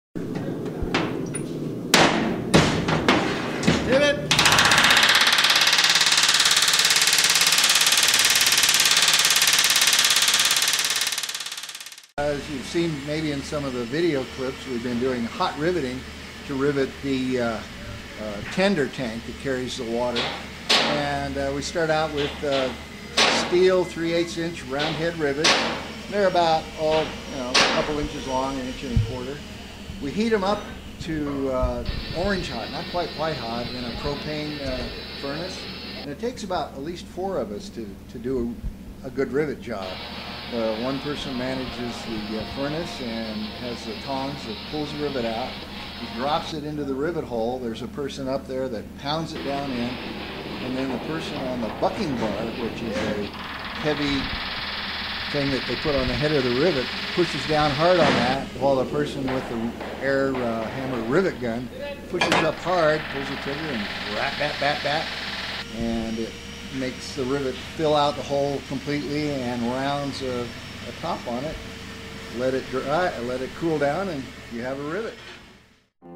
D&RGW No. 223- Riveting
The Golden Spike Chapter, Railway & Locomotive Historical Society has undertaken the project of restoring a narrow-gauged, class C-16 locomotive: Denver & Rio Grande Western No. 223. Here you can see how they have reinvented a lost art- hot riveting- to restore the tender tank.